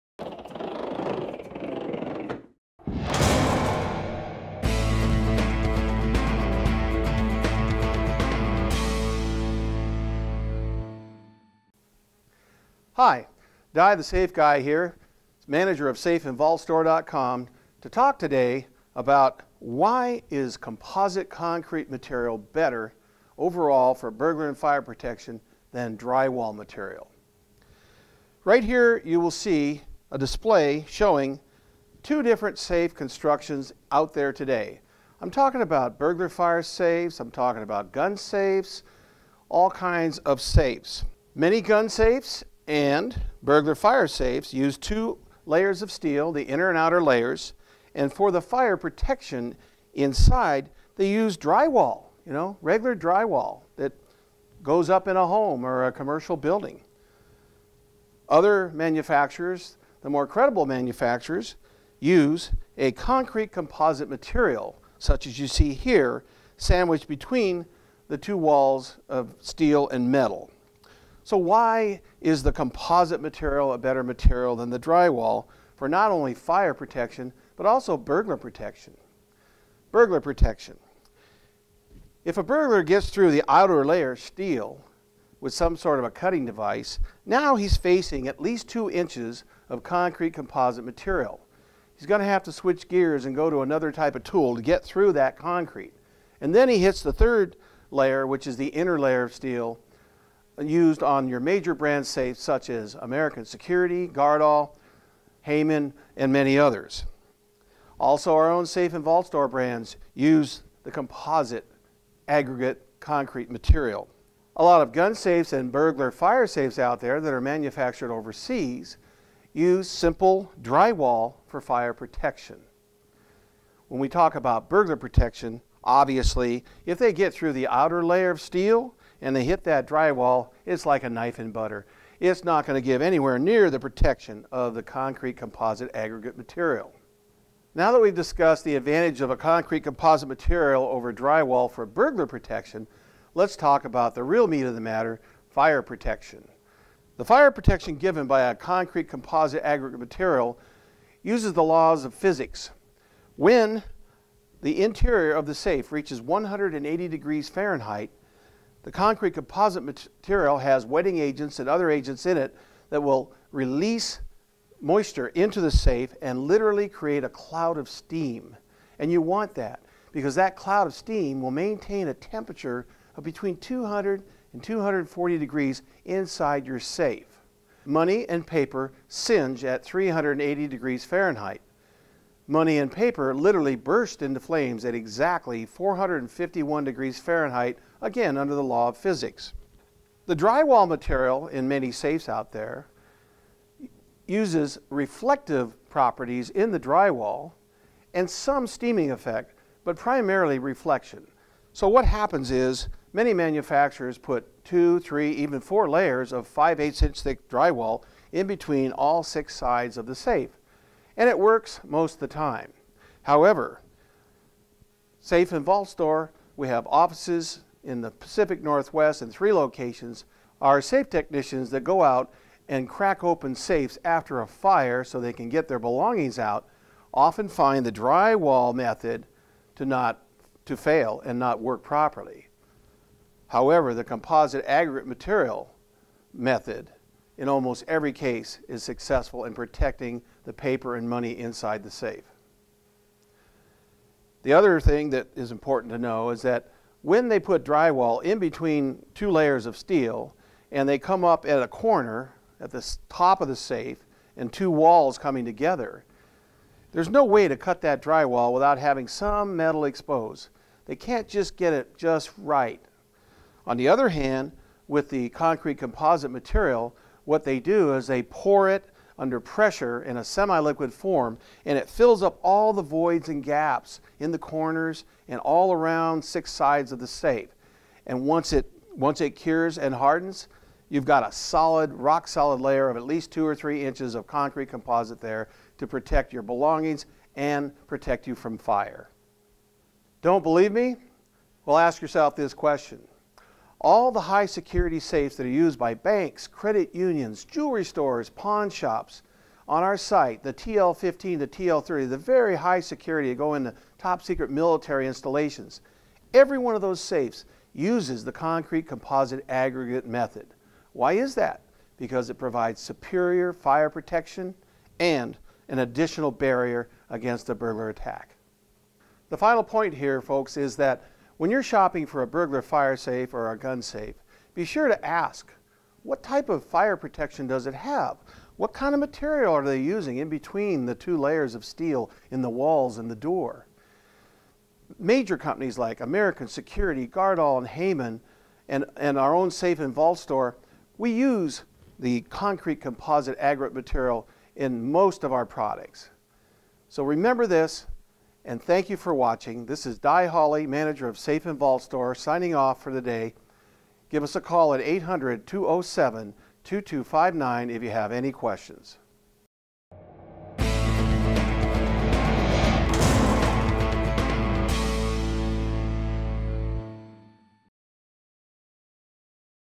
What You Should Know About Concrete Composite Material
What you should know about concrete composite material. Companies such as AMSEC use a concrete composite material (called DryLight) for fire and burglar protection. Some companies use "drywall" which offers fire protection but no burglar protection. Find out the difference between the two.